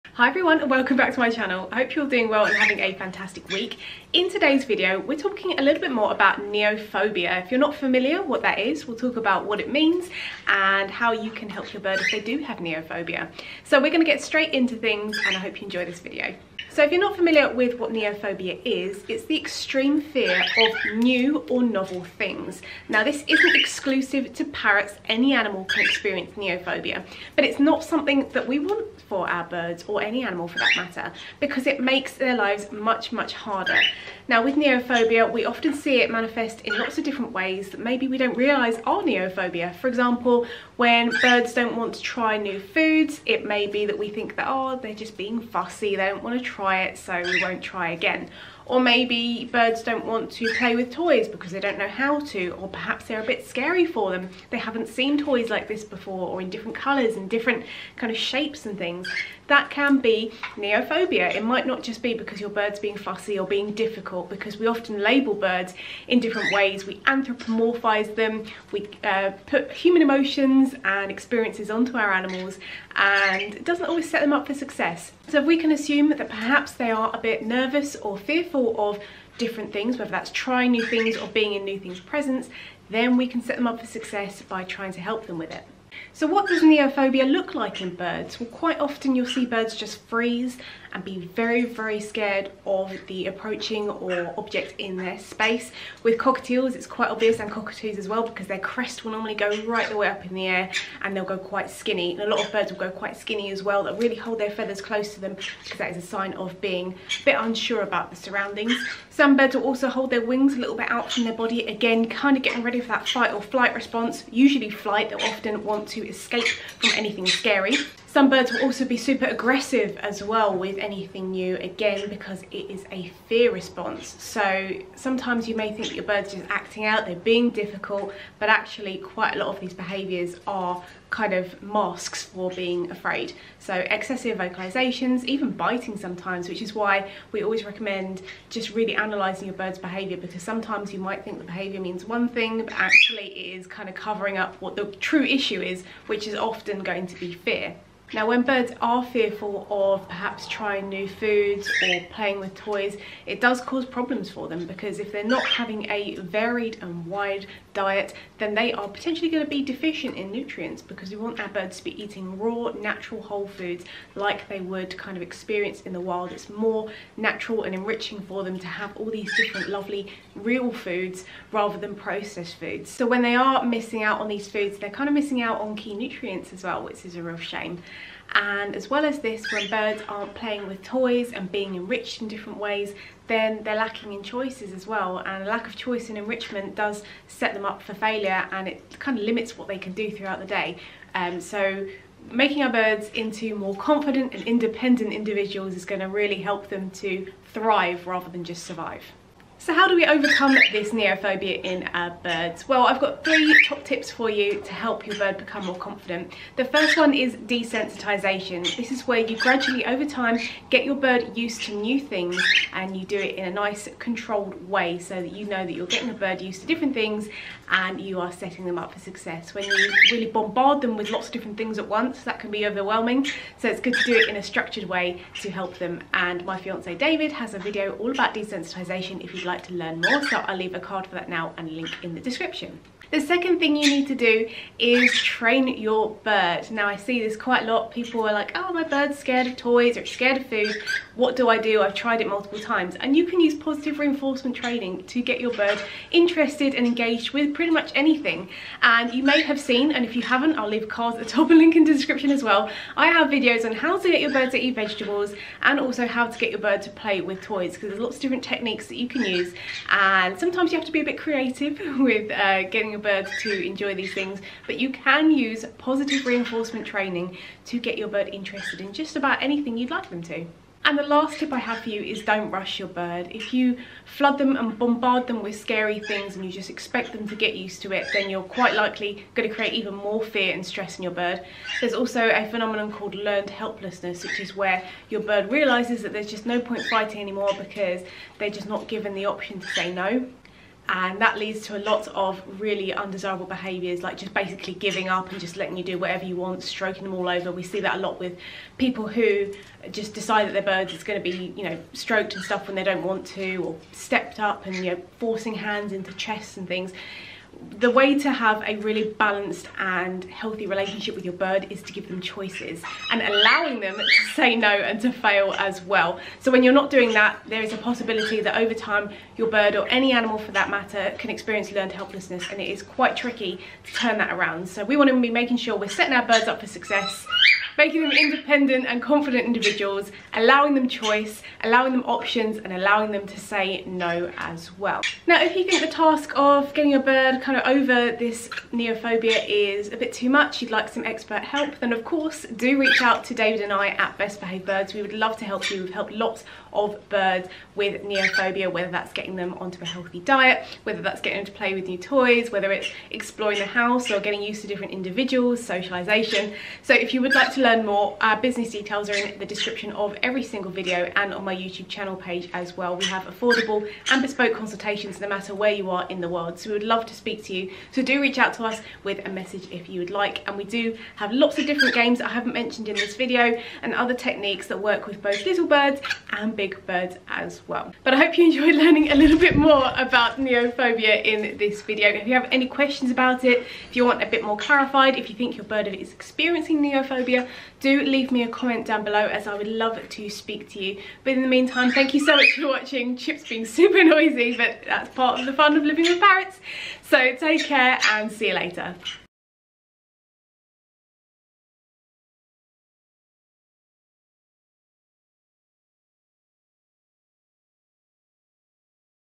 Neophobia in Parrots | BirdNerdSophie
Pet parrots can experience neophobia, even in the most loving homes. Stress and fear are very common for our pet birds, but recognising this and training them to be more confident can allow out birds to thrive, not just survive!

All profits earned through sales will go directly to the birds for their care.